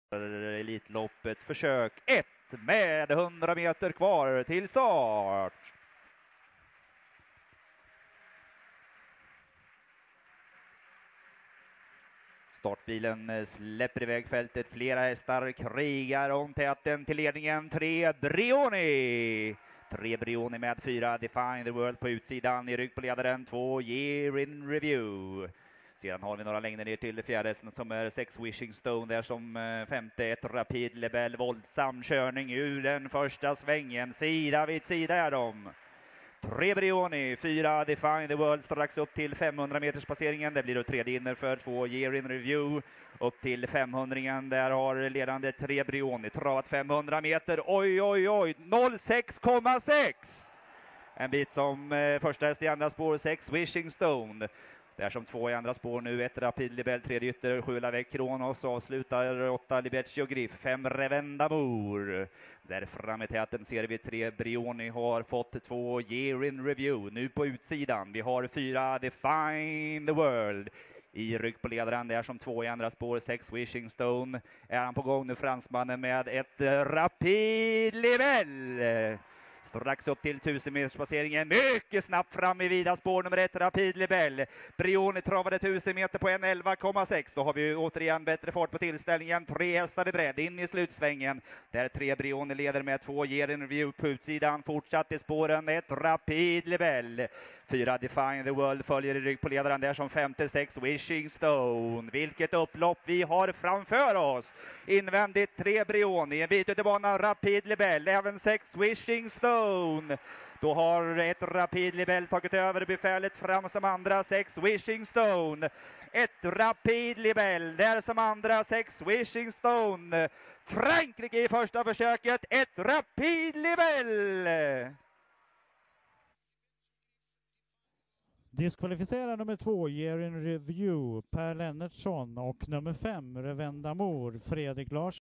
Elitloppet 2011 - Elimination 1
Lopp 6. V75-1. Elitloppet, Försök 4-åriga och äldre. 1609 m. Autostart. 8 startande. Pris: 250.000-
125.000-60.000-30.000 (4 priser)
1. Rapide Lebel* 10,3 (28) Raffin E
2. Wishing Stone* 10,7 (70) Bazire J M
3. Libeccio Grif* 10,9g (775) Smorgon M
4. Brioni* 11,2 (32) Lövgren J Opl. Lavec Kronos 12,3 (120), Define the World* dtr4 (51), Year in Review d7g (87), Reven d'Amour d8g (186). Segr. v 6 år e. Ginger Somolli-
Koranie des Noes. Äg. Prevost Alexandre, Frankrike. Uppf. Lereverend Alain, Frankrike. Trän.
Guarato Sébastien, Frankrike. 3 längder. Odds: 2,86. Plats: 1,76-2,11-5,66. Tvilling: (1/6) 11,19.
◗ Tempo: Första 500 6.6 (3 Brioni*), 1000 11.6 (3 Brioni*) sista 500 9.4

◗ Så kördes loppet: 1 RAPIDE LEBEL chanslös på ledningen och satt i tredje
ytter efter 600. Snabb attack 700 kvar, tredje spår resten, tog över i början
av upploppet, klev undan enkelt. Mäktig via 1.08,5 sista 800. 6 WISHING
STONE först i andraspår, avlöst efter 600. Satt kvar 700 kvar, loss
250 kvar, spurtade fint. 8 LIBECCIO GRIF fjärde inner, kort galopp efter
600. Loss i vida spår 200 kvar, positiv, 1.09,3 sista 600. 3 BRIONI spetsade,
tufft pressad. Fick aldrig vara ifred, slagen 200 kvar. 7 LAVEC KRONOS
backades ner i fjärde ytter. Med i tredjespår 700 kvar, trött 400 kvar. Gick
med punktering. 4 DEFINE THE WORLD pressade hårt för ledningen, rygg
ledaren efter 400 meter. Snävt loss 100 kvar, höll farten. Diskad som fyra i
mål. 2 YEAR IN REVIEW tredje inner efter 400. Fram utvändigt ledaren
1 000 kvar, slagen 300 kvar, störd till galopp 100 kvar. 5 REVEN
D'AMOUR femte ytter. Med 700 kvar, tappade travet, galopp 400 kvar.
Omstart: 7 Lavec Kronos (dålig anslutning)